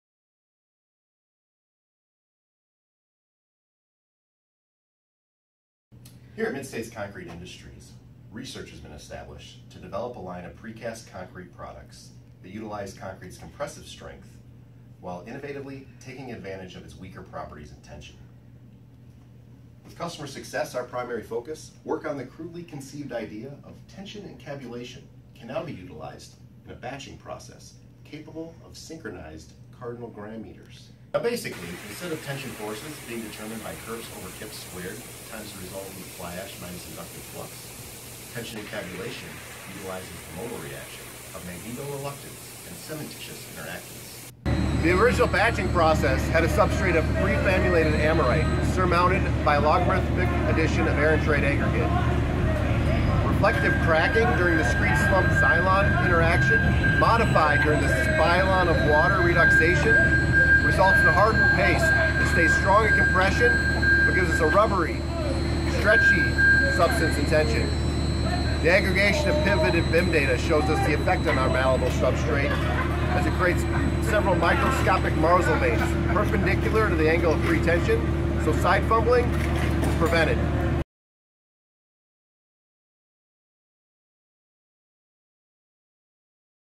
Introducing Tension Encabulation
Due to measurement errors or changing customer requirements, the length of precast hollowcore components can sometimes be wrong.

When elements are too long, the obvious solution is to cut them to length.  However, when an element is too short, precasters are forced to completely remake the elements incurring all the costs in doing so.

We’ve been diligently working on a solution to this problem and are pleased to announce through tension encabulation we are able to stretch concrete elements when field conditions dictate we do so.